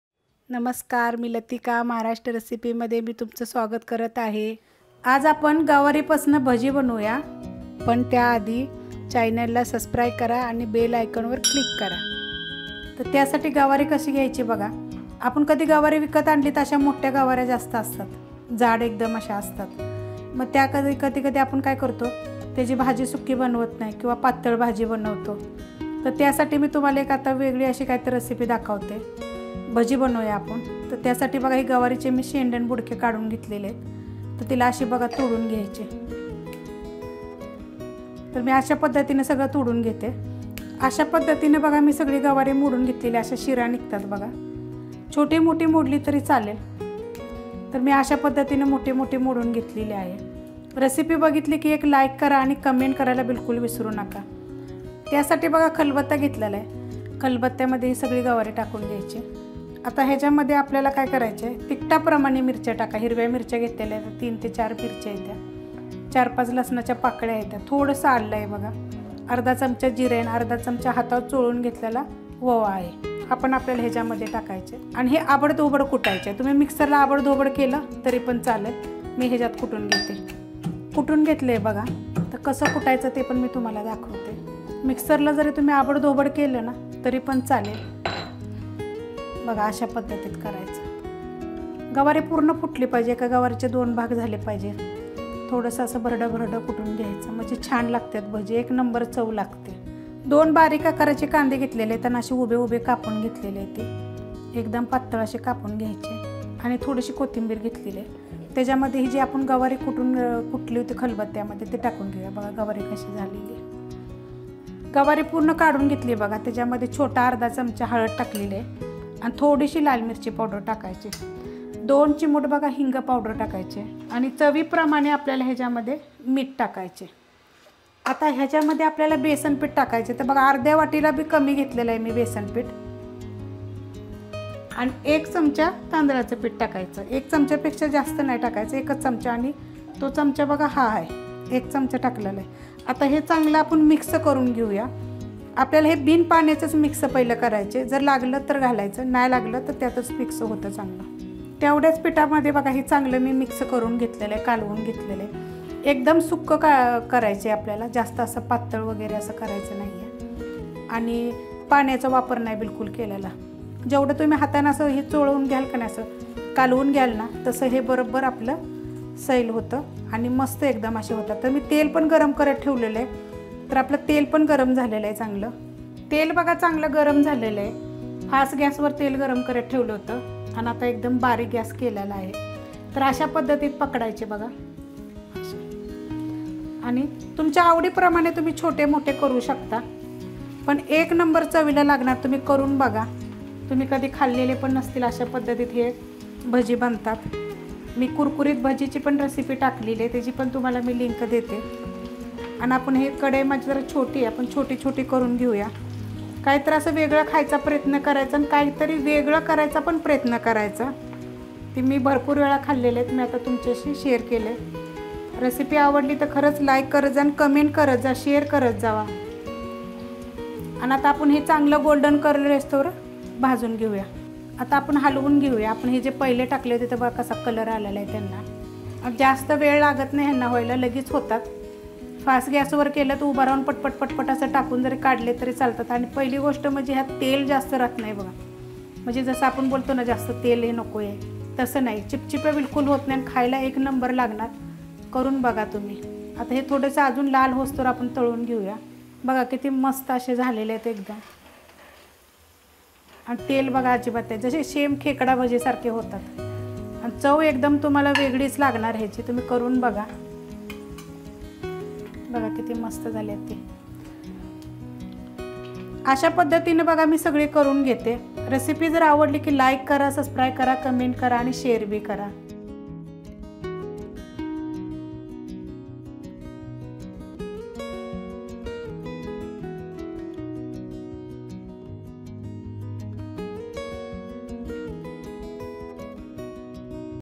अशा गवारीच्या कुरकुरीत भजी कधीच खाल्या नसतील | Gavarichi Bhaji
अशा गवरीच्या कुरकुरीत भजी कधीच खाल्या नसतील | Gavarichi Bhaji

*Ingredients :-

Cluster Bean
3-4 Green Chilli
4-5 Garlic Cloves
1/2 Inch Ginger
1/2 tbsp Cumin Seeds
1/2 tbsp Ajwain
2 Onion Chopped
Coriander Leaves
1/2 tbsp Turmeric powder
1/2 tbsp Red Chilli Powder
A Pinch Hing / Asafetida
Salt to taste
Gram Flour
1 tbsp Rice Flour